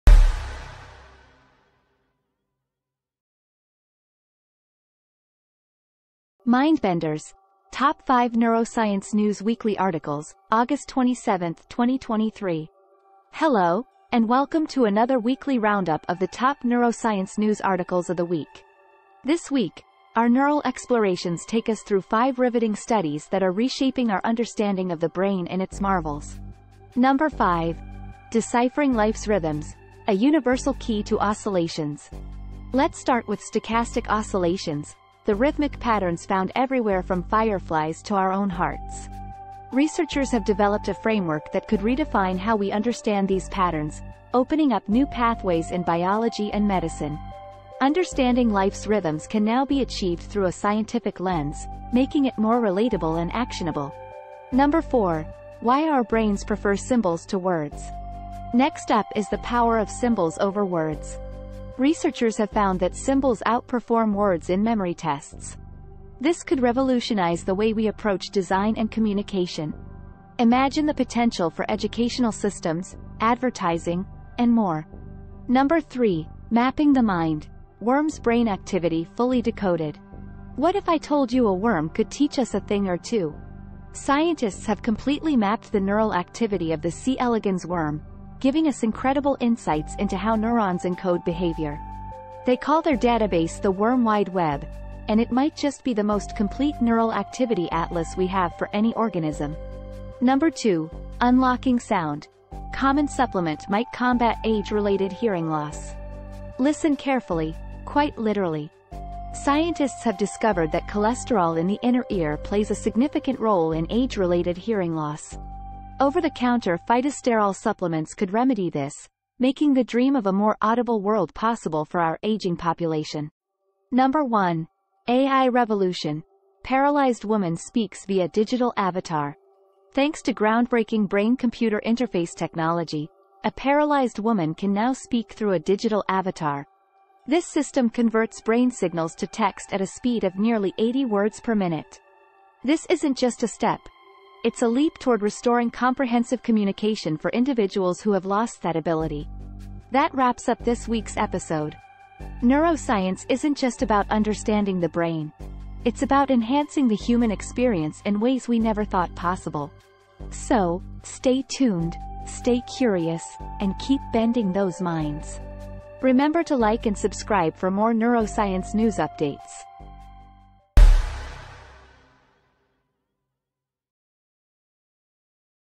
MindBenders: Top 5 Neuroscience News Weekly Articles - August 27, 2023
This week, we delve into five groundbreaking studies in neuroscience that are shattering our previous notions about life's natural rhythms, human communication, and more.

From the neural activity of worms to over-the-counter solutions for age-related hearing loss, these discoveries are redefining our understanding of biology, medicine, and what it means to be human.